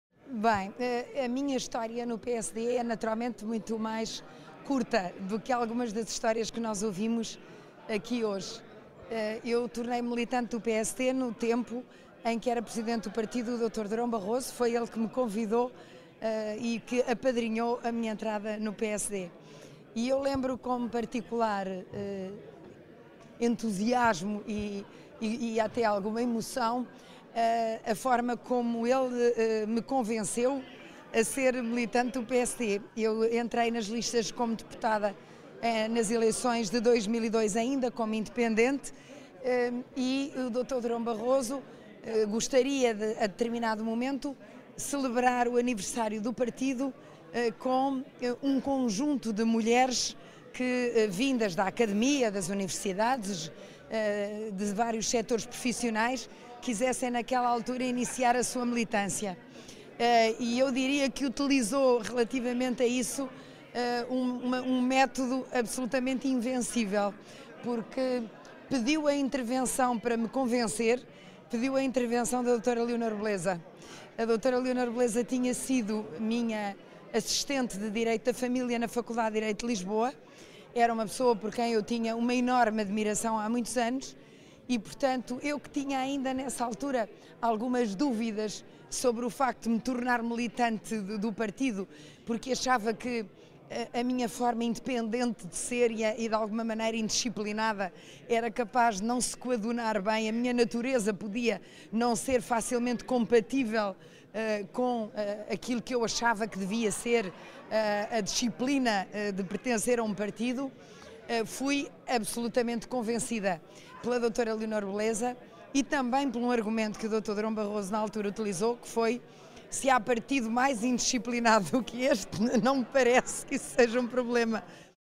"De independente a militante num partido indisciplinado"
Foi Durão Barroso (com a ajuda de Leonor Beleza) que convenceu Teresa Morais a filiar-se no PSD. O argumento do ex-líder foi forte: para a rebeldia da independente, um partido indisciplinado.